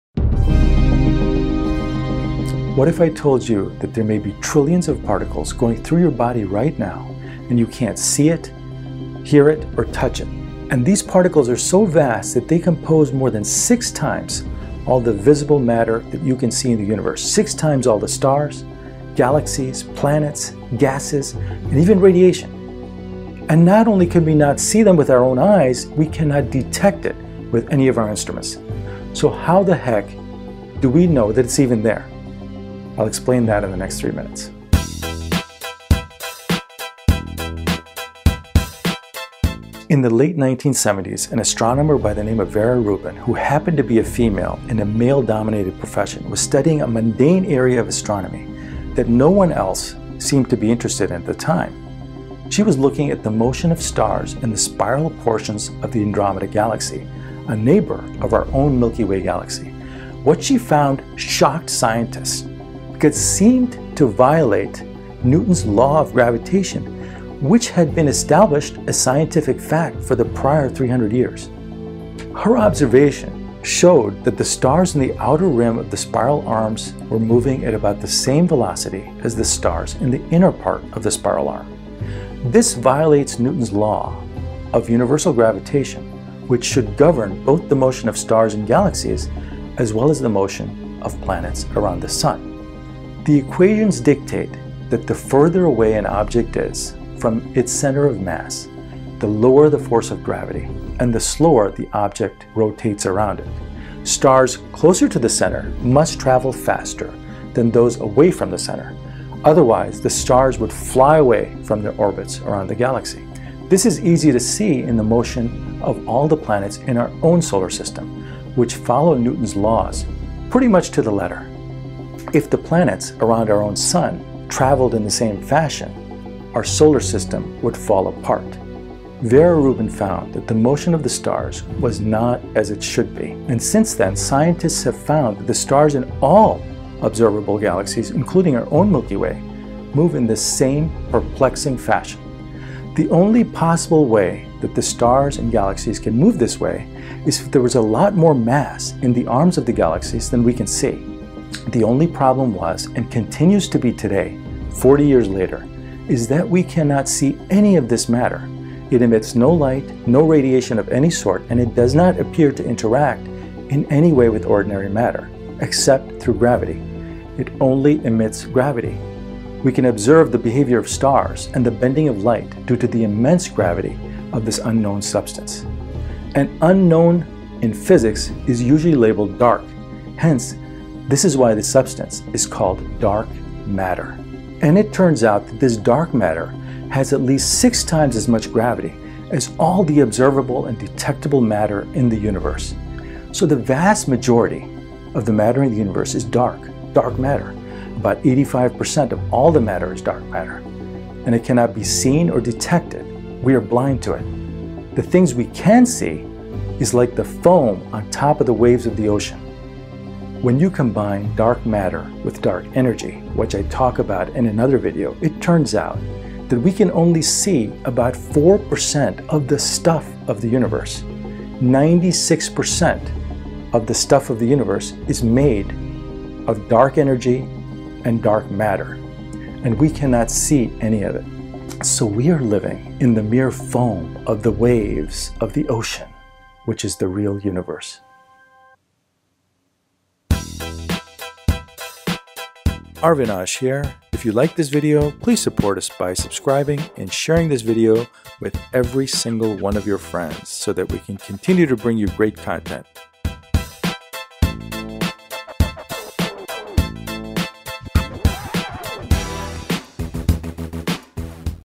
What Is Dark Matter?
What is Dark Matter? -- What if I told you that there may be trillions of particles going through your body right now, and you can’t see it, hear it, or touch it.

And that these particles are so vast, that they compose more than 6 times all the visible matter you can see in the universe.

6 times all the stars, galaxies, planets, gases, and radiation.

And we not only cannot see any of it with our eyes, we cannot detect it with any of our instruments.

So how the heck do we know that it’s even there?

In the late 1970’s, an astronomer by the name of Vera Rubin, who happened to be a female in a male-dominated profession, was studying a mundane area of astronomy that no one else seemed to be interested in at the time.

She was looking at the motion of stars in the spiral portions of the Andromeda galaxy, a neighbor of our own milky way galaxy.

What she found shocked scientists…because it seemed to violate Newton’s laws of gravitation which had been established as scientific law for the previous 300 years.

Her observations showed that the stars in the outer rim of the spiral arms were moving at about the same velocity as the stars in the inner part of the spiral arm.

This violates Newton’s law of universal gravitation which should govern both the motion of stars in galaxies as well as the motion of planets around the sun.

Force of Gravity = G m1 m2 / r2
m1= mass of first planet
m2 = mass of second planet
r2= square of distance between them
G = Gravitational constant

The equations dictate that the further away an object is from the center of mass (r), the lower the force of gravity, and the slower the object rotates around it.

Stars closer to the center must travel faster than those away from the center.

Otherwise, the stars would fly away from their orbits around the galaxy.

This is easy to see in the motion of all the planets in our own solar system – which follow Newton’s laws pretty much to the letter.

Vera Rubin found that this was not the case with the stars in the Andromeda galaxy.  And since then, scientists have found that the stars in all observable galaxies, including our own milky way, move in the same perplexing fashion.

The only possible way that stars in galaxies can move this way is if there was a lot more mass on the arms of the galaxies than we can see.

Vera Rubin’s observations confirmed an earlier 1933 hypothesis proposed by Swiss-American astronomer Fritz Zwicky that the universe contains a lot more mass than we can see with our telescopes.

The only problem was, and continues to be today, 40 years later, is that we cannot see any of this matter, it emits no light, no radiation of any sort, and it does not appear to interact in any way with ordinary matter except through gravity.

It only emits is gravity.  We can observe the behavior of stars and the bending of light due to the immense gravity of this unknown substance.

An “unknown” in physics is usually labeled Dark.  Hence this is why this substance is called “Dark Matter”

And it turns out that this dark matter has at least 6 times as much gravity as all the observable and detectable matter in the universe.

So the vast majority of the matter in the universe is dark matter – about 85%.  And it cannot be seen or detected (except by its gravity).

The things we CAN see is like the foam on top of the waves of the ocean, but we are not able to see waves or the ocean itself.  We and our instruments are blind to it.

We are living in the mere foam of the waves on the ocean which is the real universe.